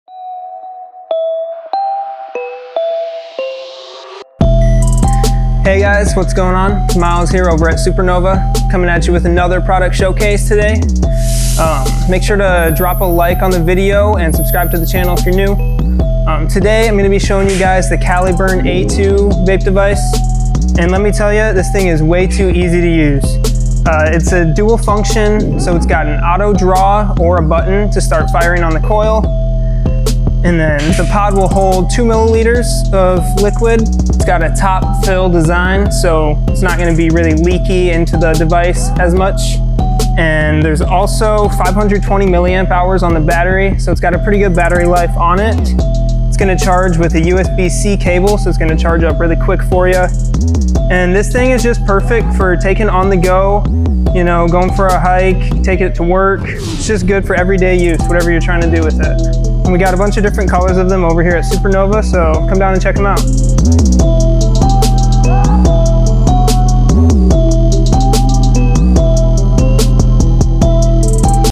1min Video Spot Production - Caliburn A2 Pod System
New Video Production for SuperNova Smoke Shop in San Antonio Tx.